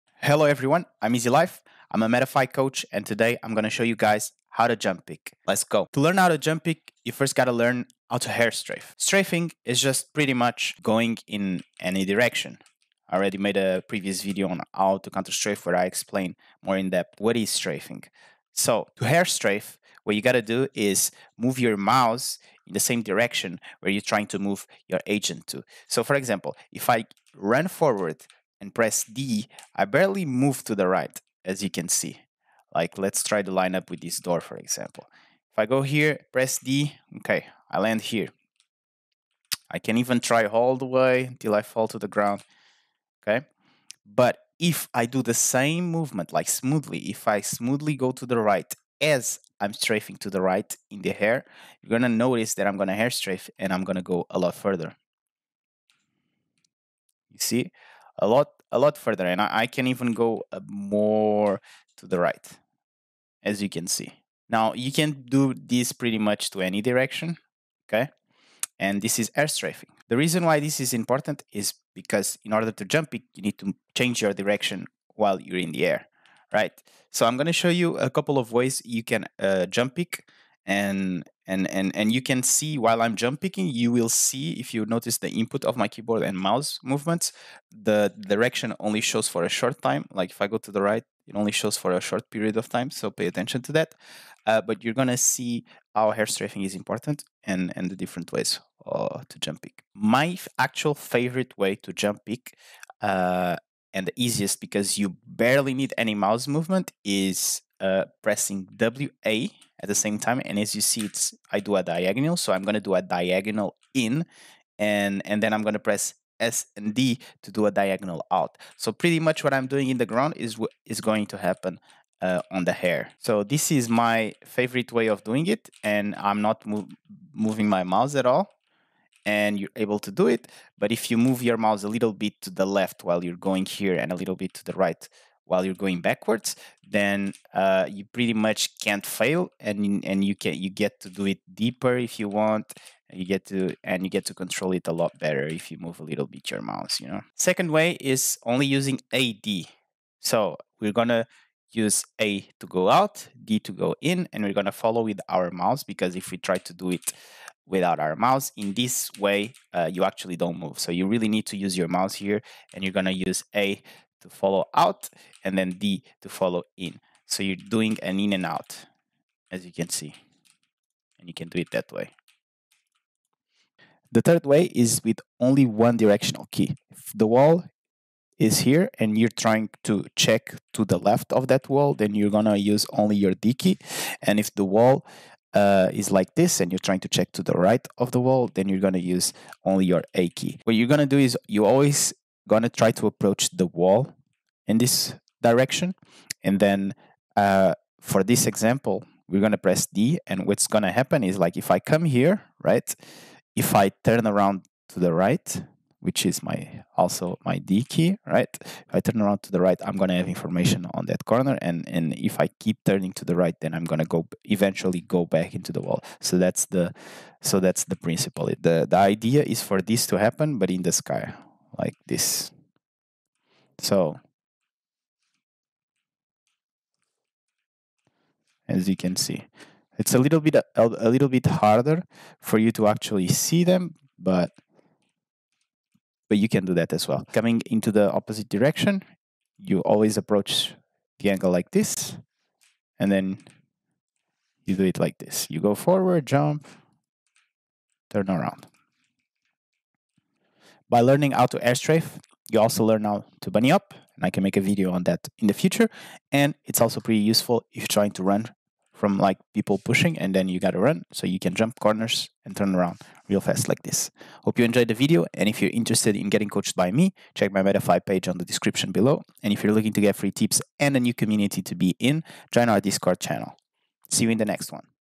How to Jump Peek (Movement Guide)
I show you guys different ways to jump peek, this is a basic movement guide!